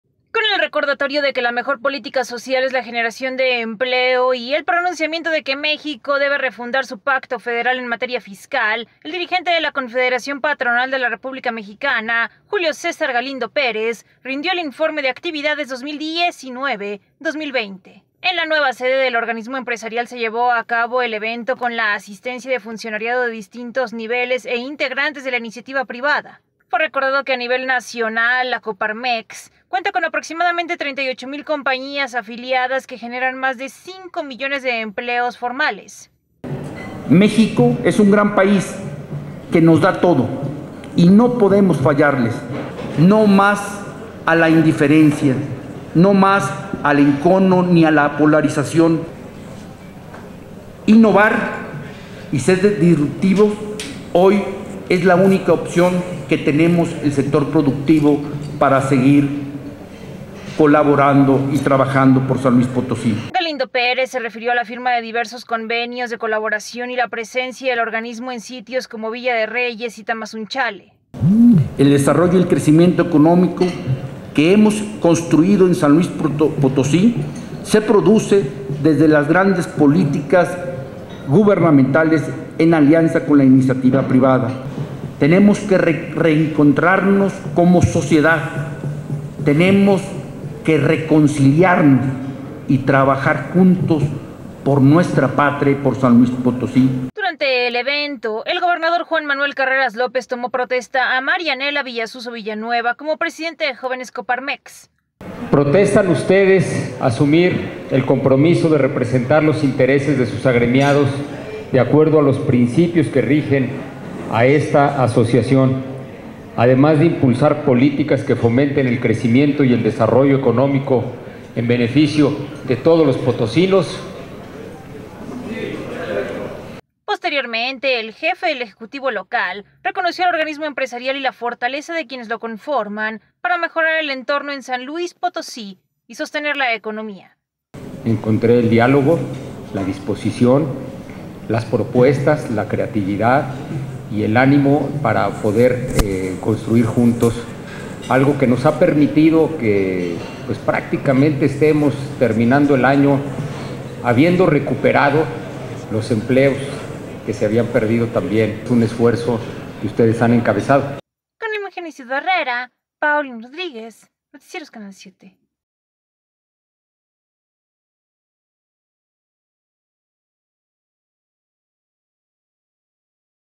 La Coparmex rindió el Informe de Actividades 2019-2020.
SLP.- Con el recordatorio de que la mejor política social es la generación de empleo y el pronunciamiento de que México debe refundar su pacto federal en materia fiscal, el dirigente de la Confederación Patronal de la República Mexicana (COPARMEX) Julio César Galindo Pérez rindió el informe de actividades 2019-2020.

En la nueva sede del organismo empresarial se llevó a cabo el evento, con la asistencia de funcionariado de distintos niveles e integrantes de la iniciativa privada.

Galindo Pérez se refirió a la firma de diversos convenios de colaboración y la presencia del organismo en sitios como Villa de Reyes y Tamazunchale.

Durante el evento el gobernador, Juan Manuel Carreras López tomó protesta a Marianela Villasuso Villanueva como presidente de Jóvenes COPARMEX.

Posteriormente, el Jefe del Ejecutivo Local reconoció al organismo empresarial y la fortaleza de quienes lo conforman, para mejorar el entorno en San Luis Potosí y sostener la economía.